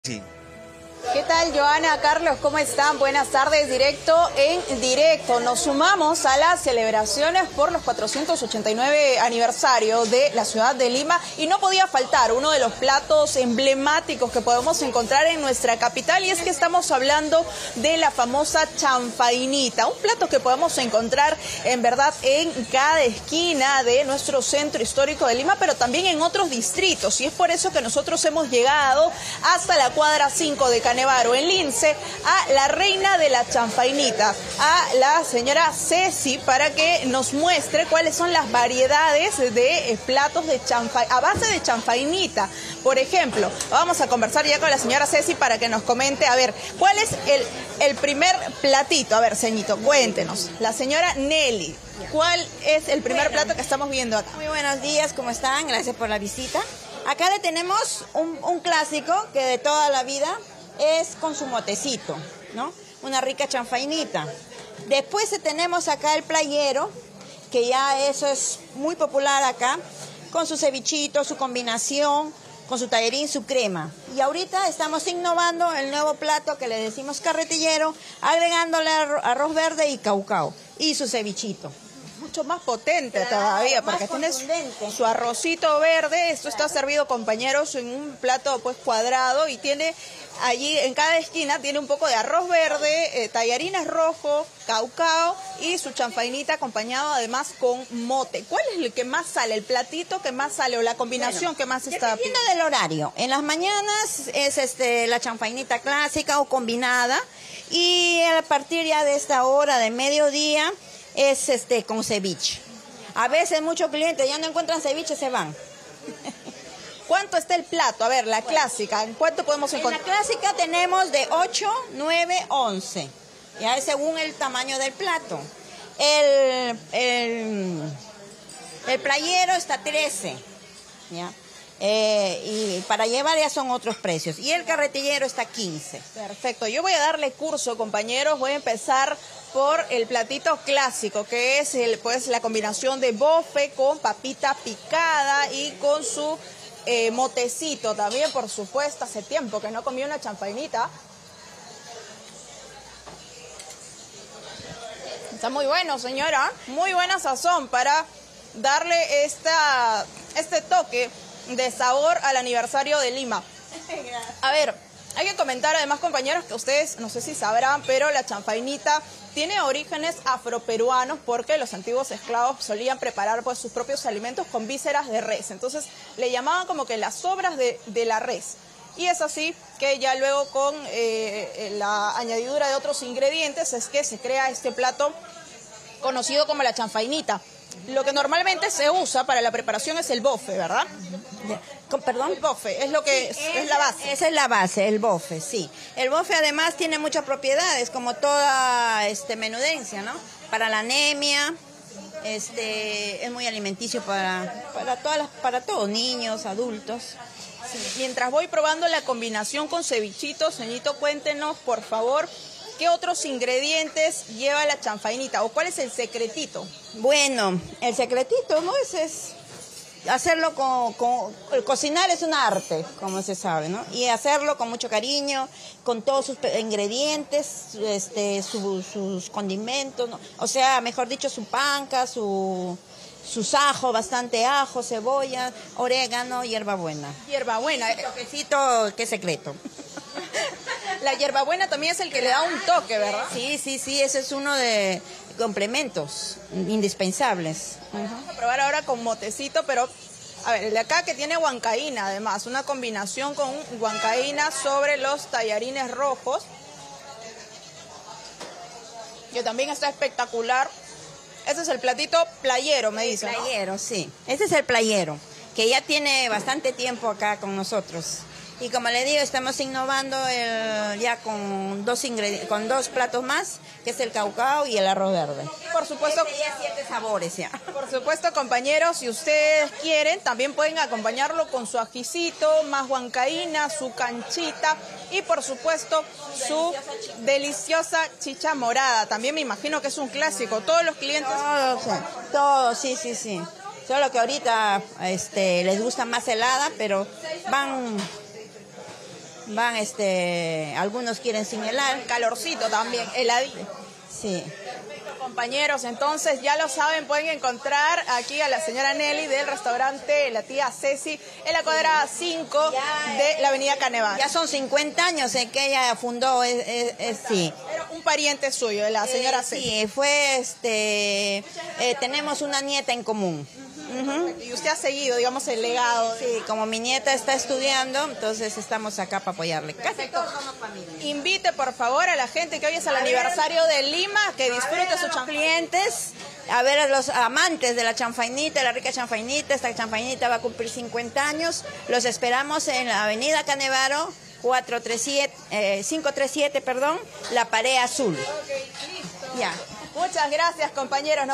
Aniversario de Lima: La chanfainita, uno de los platos limeños más populares
🔴 Probamos uno de los platos limeños más populares, la chanfainita. En la cuadra 5 de la Av. Canevaro conocemos a la 'reina de la chanfainita'.